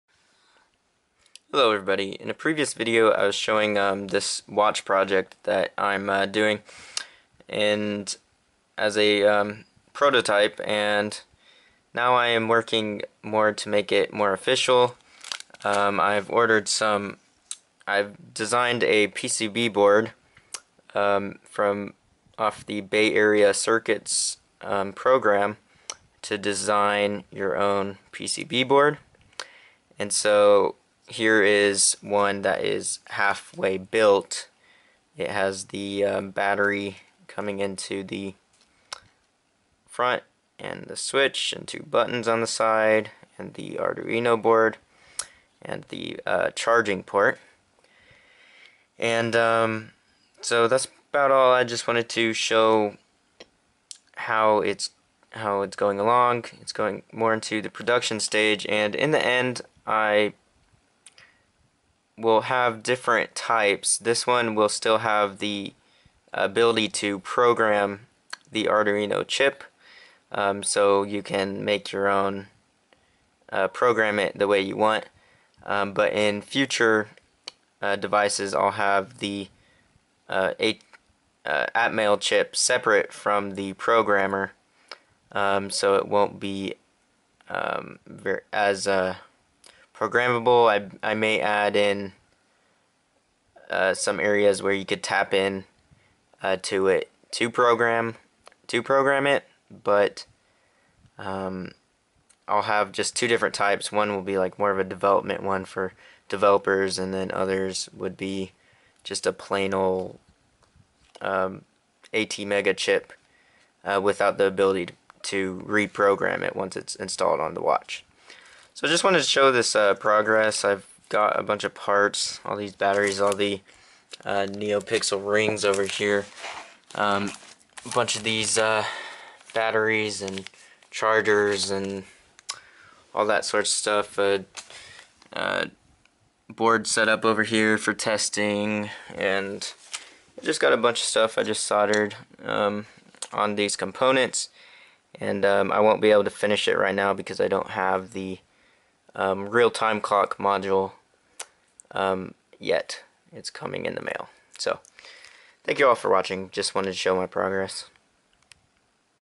Arduino Smart Watch Project Update 1
Just an update on my project I've got going. Let me know what you think! Got an ATtiny85 on a Digispark, designed a pcb board, and am starting to get more into the production stage. Previous video